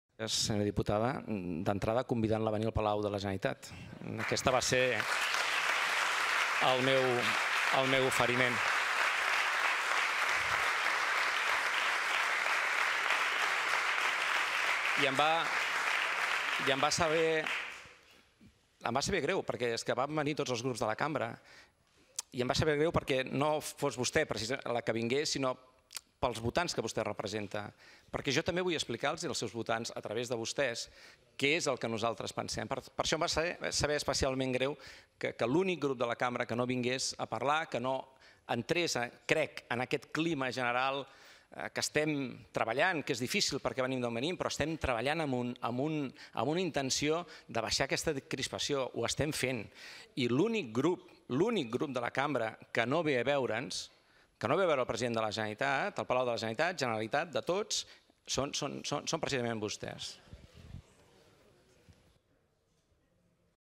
Discurs del president Quim Torra - 20.06.2018
Pregunta al president de la Generalitat a respondre oralment en el Ple del Parlament sobre la manera d'afrontar la fractura social